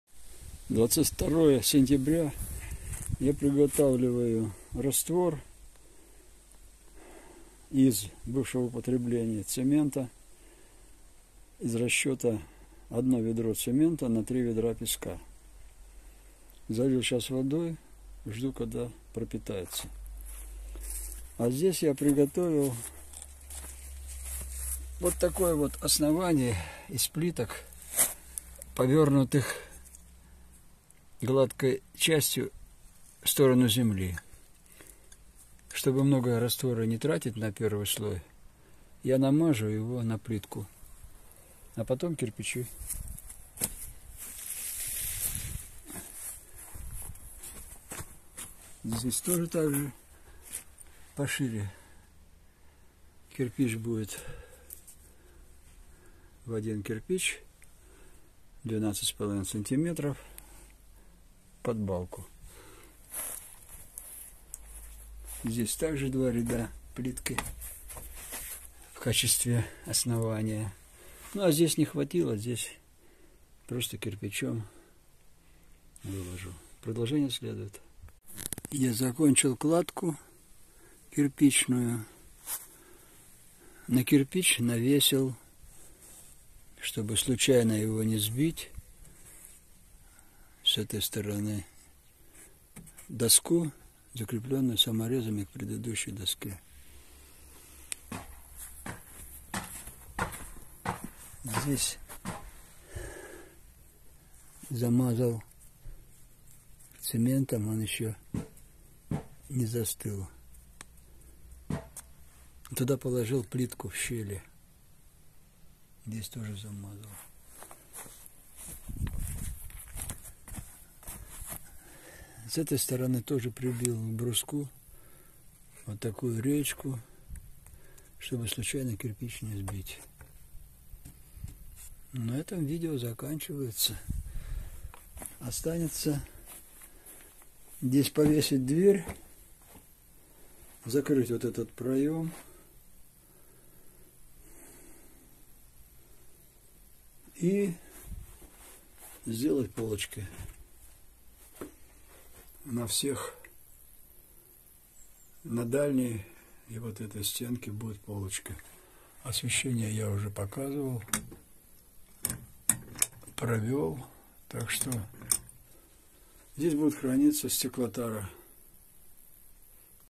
Кирпичный ЛЕНТОЧНЫЙ низ между столбами .
Нижнюю часть стен сарая я выложил для надёжности и зашиты от влаги из кирпича!